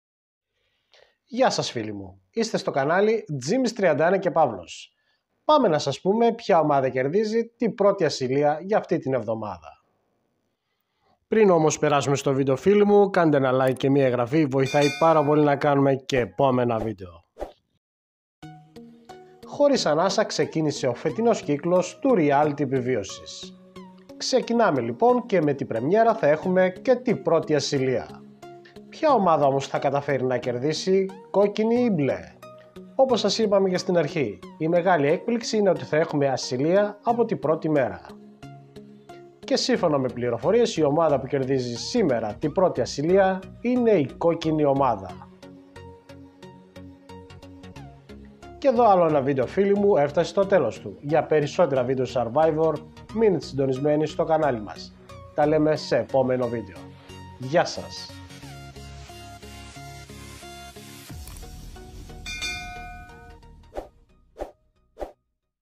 Survivor Spoiler: Αυτή η ομάδα κερδίζει τη 1η ασυλία σήμερα!
Ξεκινάμε λοιπόν! To survivor all star κάνει πρεμιέρα.

H πρώτη ασυλία σήμερα. Ποια ομάδα θα κερδίσει; Κόκκινοι ή μπλε;

Χωρίς ανάσα θα ξεκινήσεο ο φετινός κύκλος του ριάλιτι επιβίωσης.

Οι παίκτες αφού χωριστούν σε ομάδες αναμένεται να τα δώσουν όλα για τις πρώτες πολύ σημαντικές νίκες.

Πιο συγκεκριμένα κόκκινοι και μπλε θα παλέψουν για την πρώτη ασυλία.

όπως είπαμε η μεγάλη έκπληξη είναι γεγονός.

Ασυλία από την πρώτη ημέρα.

Ποια ομάδα θα κερδίσει;;

Σύμφωνα με πληροφορίες σήμερα κερδίζει η κοκκινη ομάδα.